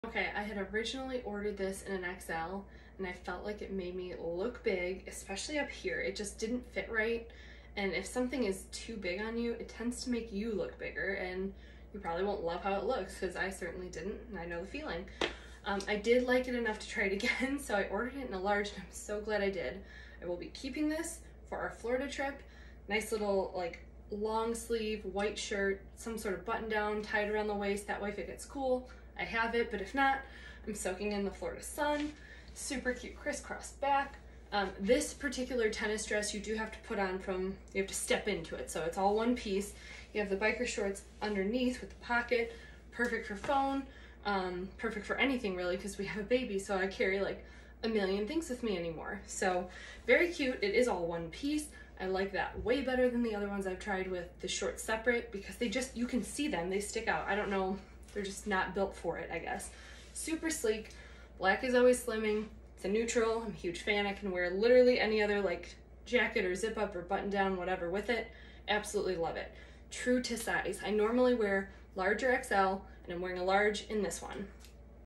Amazon Tennis Dress with Built-in Bra & Shorts Review
Let's call this what it is, it's a Halara dupe. I haven't tried the Halara Wannabe Everyday tennis dress simply because it's not on Amazon and I can seemingly never nail down sizing the first try on non-universal pieces. But this dress is awesome! I did get the XL first but of course, ultimately needed a large. I'd call this a true-to-size fit, don't size up or the built-in bra won't be the support you need!

Black Tennis Dress with Built-in Bra & Shorts details:
- 9 colors/styles to choose from, mine is Black
- Six different sizes
- Single bodysuit design keeps everything smooth and leaves you feeling confident in the dress
- Style with a flannel around the waist, wearing a button down, throw on a belt bag and some white sneakers and you're good to go!
- Follow my 30 Days of Amazon Fashion series to see this styled live later this month (will be easily located in the Guides tab on my page under the 30 Days of Amazon Fashion guide)
- I normally wear L/XL and wear an L in this dress